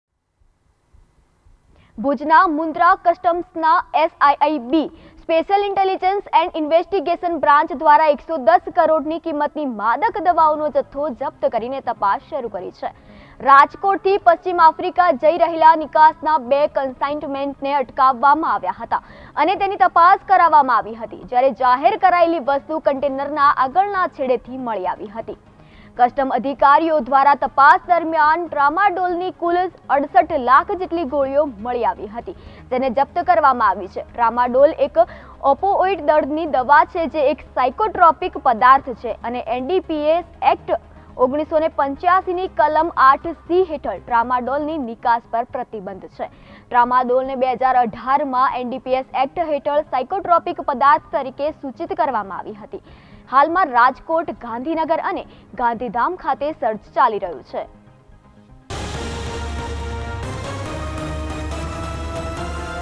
Kutch: મુન્દ્રામાં 110 કરોડનો માદક દવાનો જથ્થો જપ્ત ।। PTN News Media ।।
મુન્દ્રામાં 110 કરોડનો માદક દવાનો જથ્થો જપ્ત
SIIB દ્વારા જથ્થો જપ્ત કરી તપાસ શરૂ કરી
રાજકોટથી પશ્ચિમ આફ્રિકા જતાં બે કન્સાઈનમેન્ટ અટકાવાયા
ટ્રામાડોલની કુલ ૬૮ લાખ જેટલી ગોળીઓ મળી